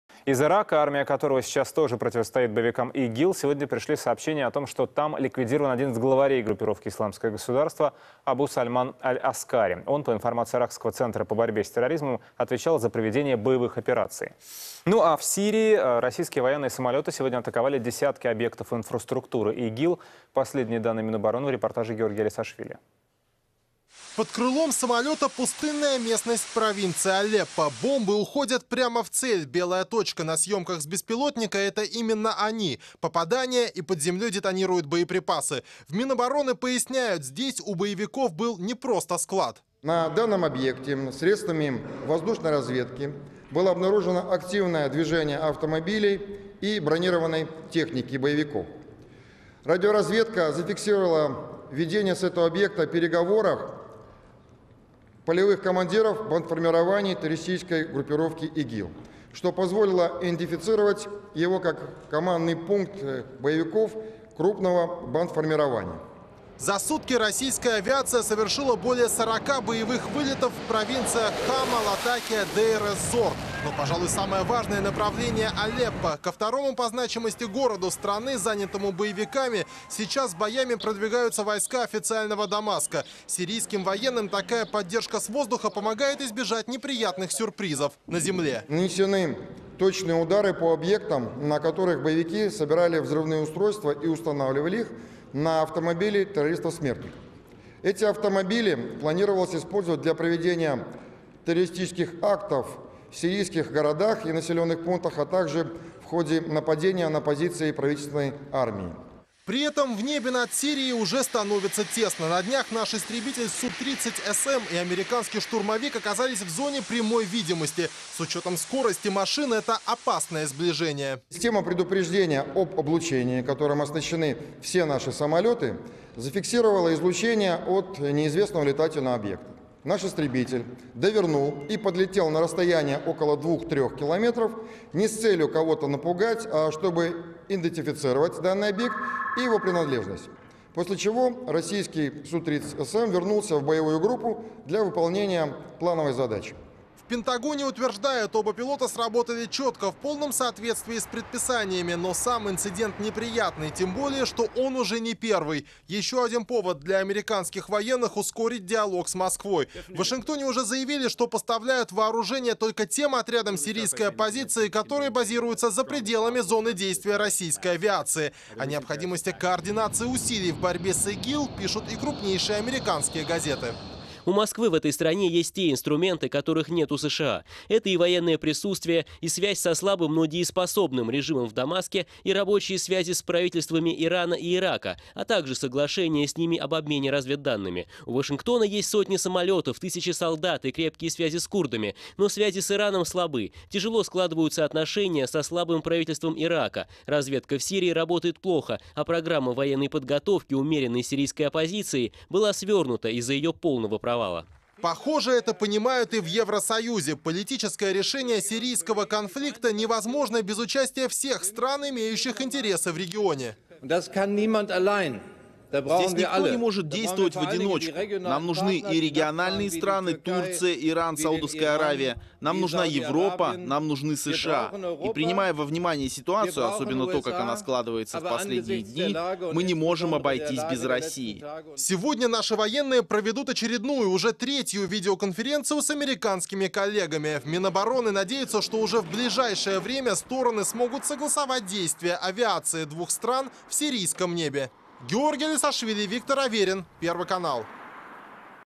Российские военные самолёты сегодня атаковали десятки объектов инфраструктуры ИГИЛ
минобороны, ввс, сирия, игил